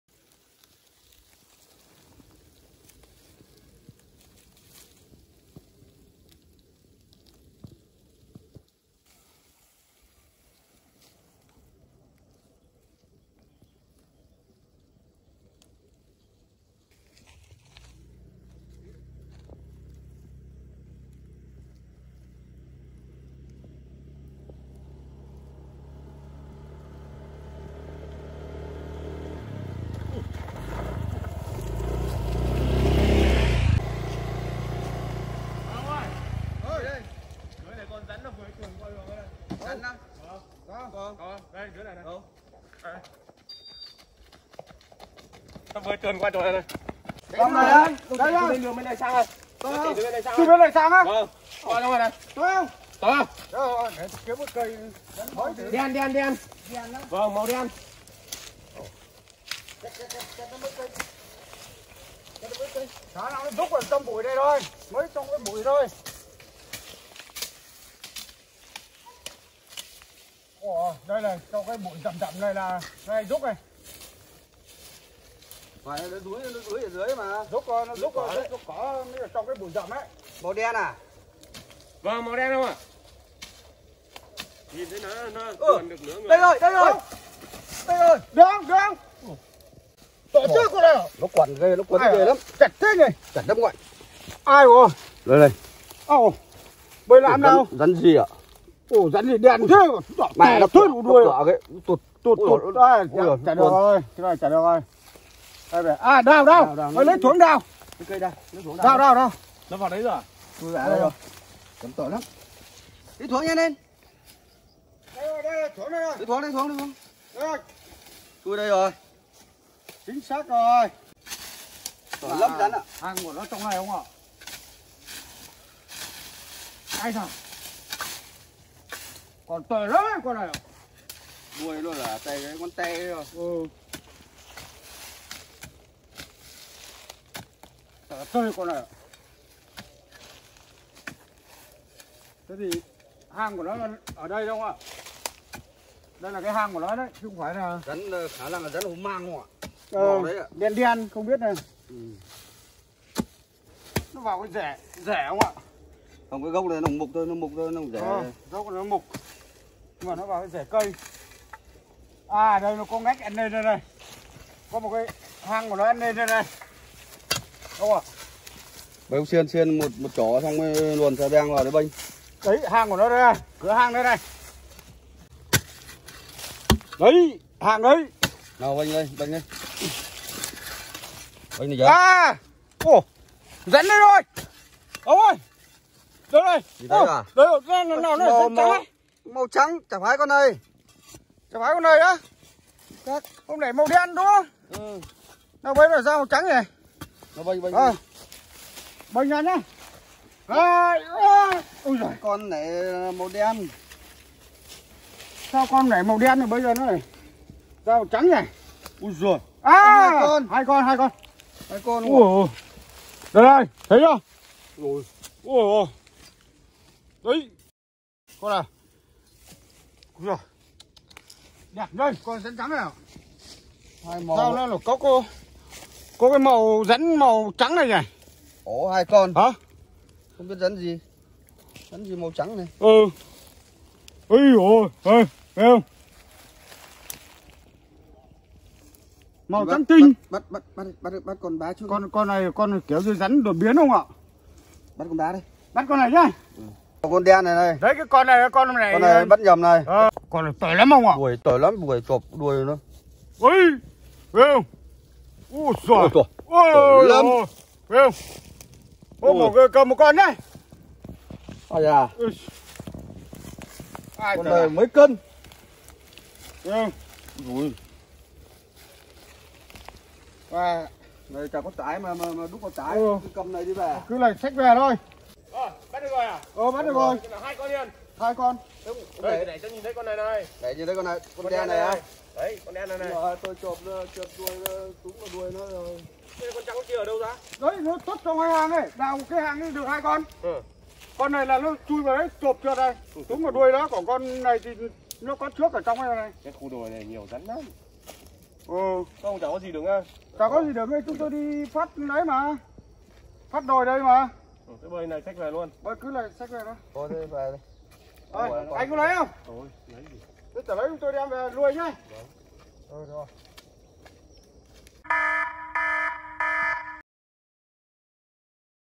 Rắn hổ mang bò ra đường sưởi ấm và cái kết gặp ngay bác thợ săn | Snake hunter
- Nếu các bạn nhìn thấy rắn thì đừng tự ý bắt mà hãy gọi những chuyên gia có kinh nghiệm nhé.